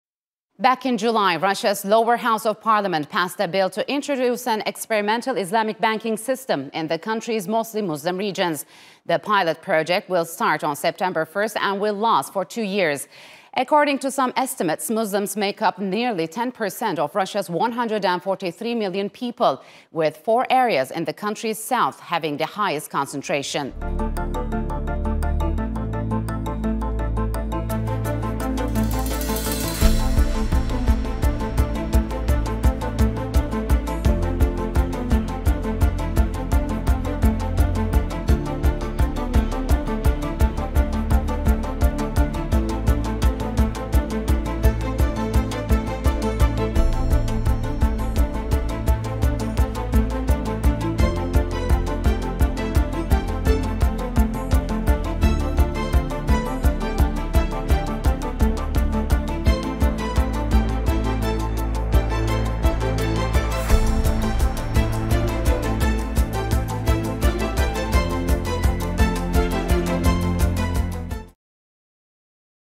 RUSSIA TO INTRODUCE THE ISLAMIC BANKING SYSTEM
Russian parliament greenlights Islamic banking in four predominantly Muslim-populated regions and starts on September 1, 2023. 🇷🇺